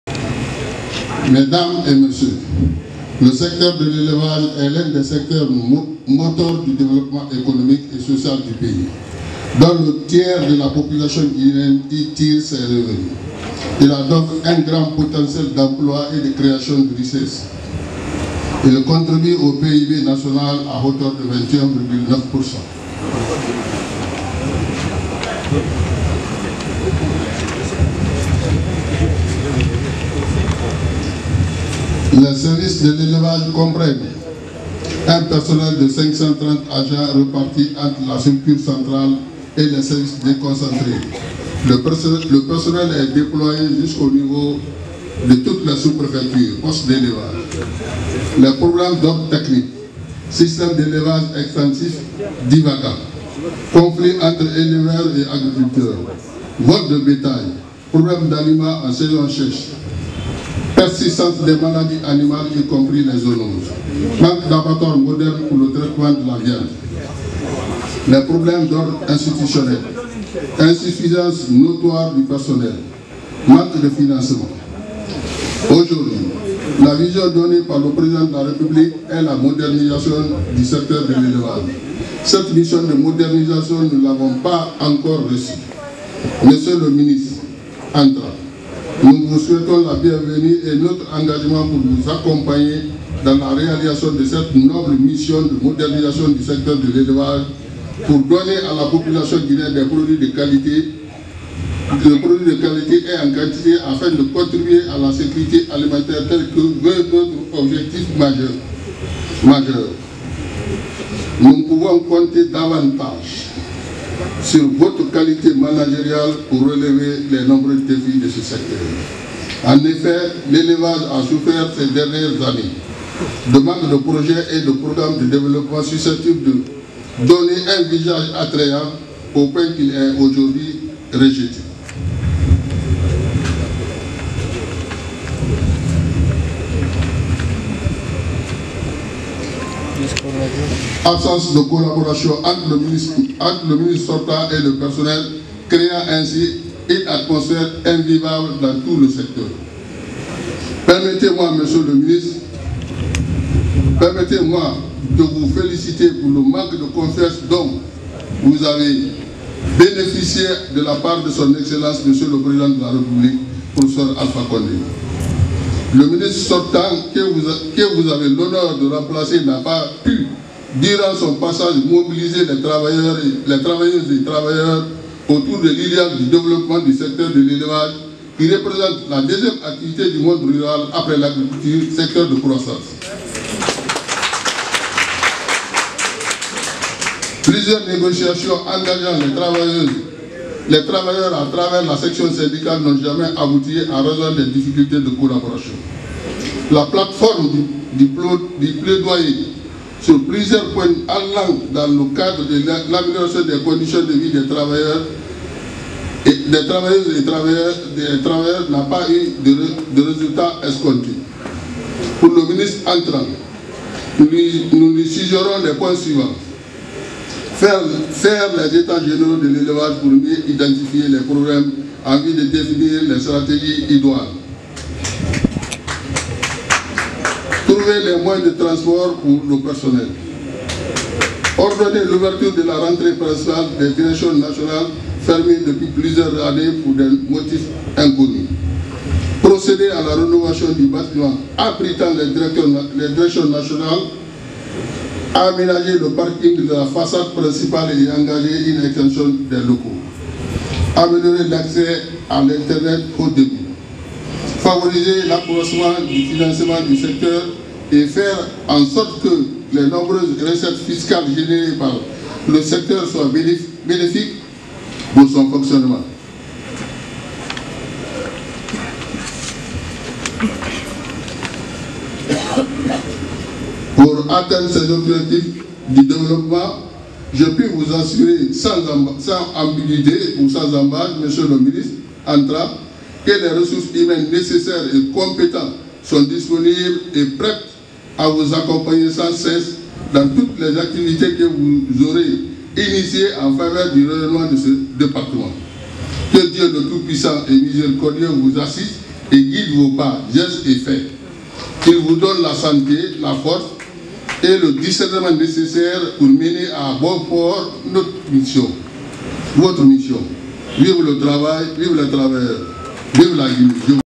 Élevage : Dr. Karim Bangoura s'attaque à la gestion de Mohamed Tall
Elevage : Dr. Karim Bangoura, secrétaire général de la section syndical, s'attaque à la gestion de Mohamed Tall